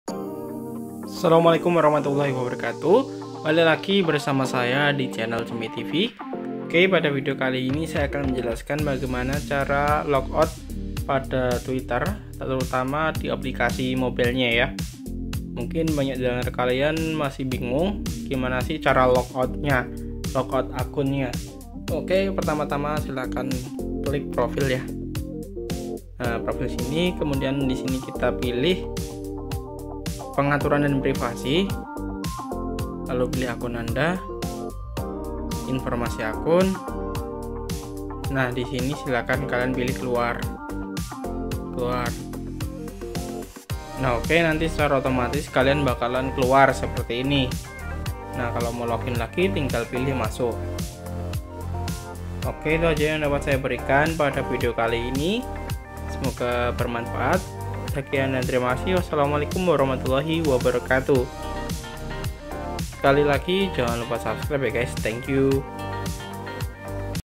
Cara Logout Akun Di Aplikasi Twitter dengan Mudah
Pada video kali ini saya akan menjelaskan bagaimana cara logout akun twitter di aplikasinya dengan mudah. Semoga bermanfaat.